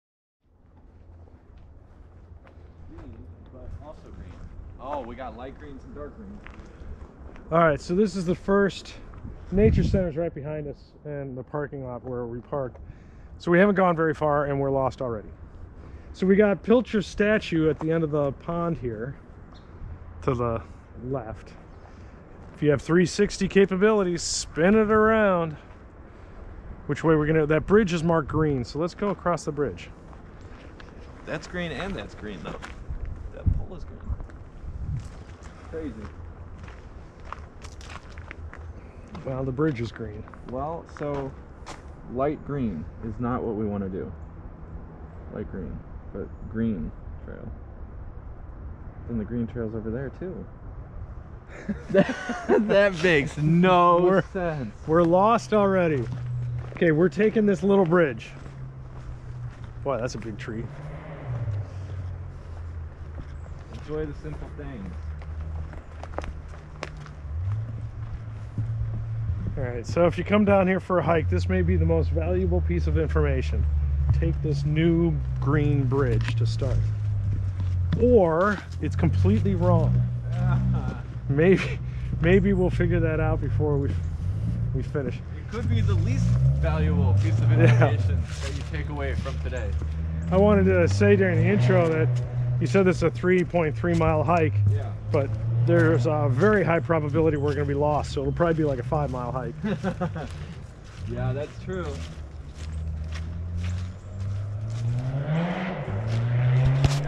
Pilcher Park Nature Center, Joliet IL - Pilcher Statue (Hike360° VR Video)
We walked West toward the Pilcher Statue and the bridge over the stream. After that? We get seriously lost while still in sight of the Nature Center Building. The problem that we found was that there are too many trails and the map doesn't help.